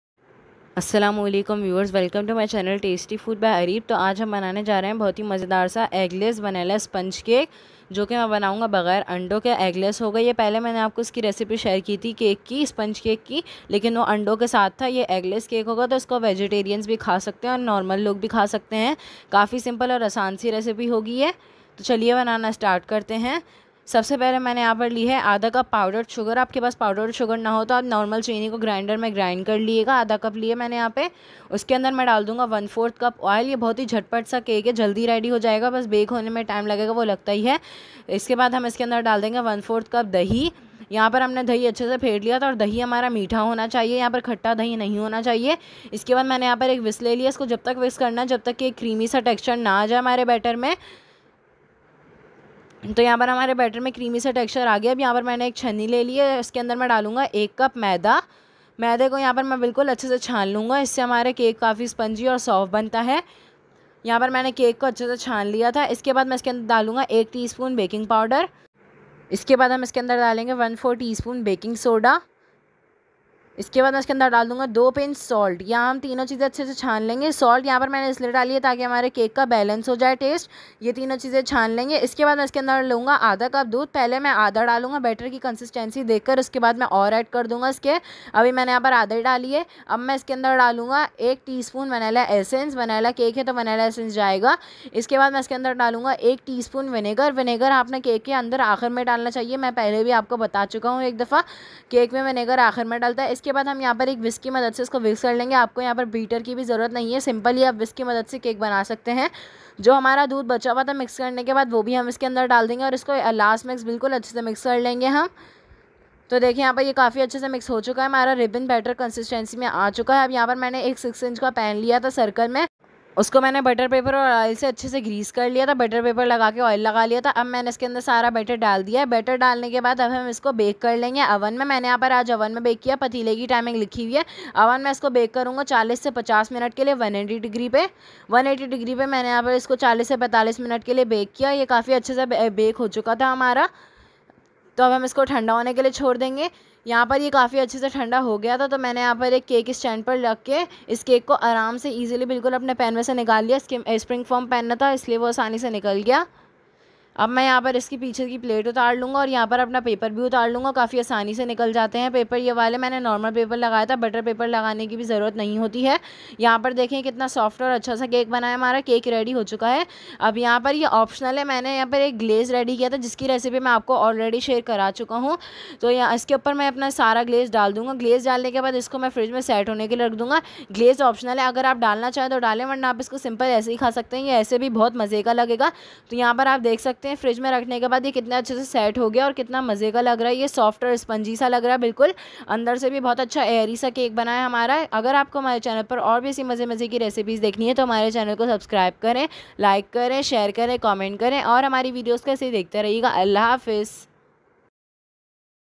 eggless vanilla sponge cake recipe without oven | Dessert recipes
eggless vanilla sponge cake recipe by Tasty Food By Areeb

today I make very simple and easy eggless cake   which is super yummy to eat and very easy to make i made this cake in an hour it is spongy moist and fluffy.  without egg I made this cake. I decorate it with glaze if you don't want glaze you can eat it simple it taste delicious.